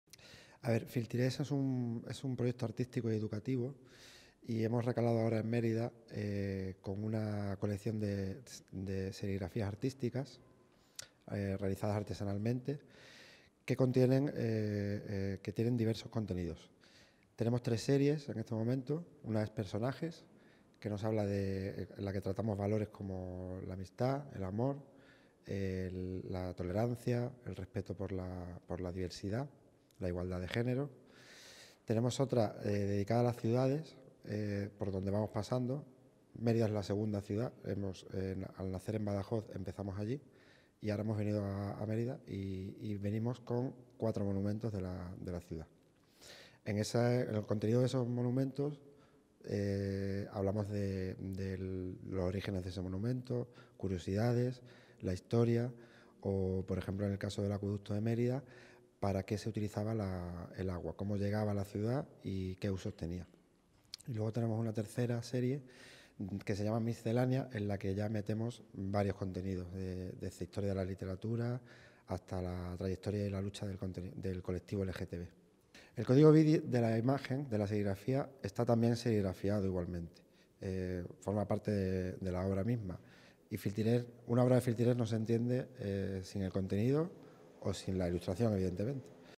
Una exposición aúna ilustraciones y relatos cortos en Mérida
La exposición 'Filtirés. Todo lo que sentimos está conectado' muestra en Mérida serigrafías en blanco y negro que a lo largo de distintas series muestra una producción artística que aúna ilustraciones y relatos cortos e hitos históricos a los que se accede a través de un código bidi. –Chesku Jiménez, uno de sus creadores, habla sobre la misma.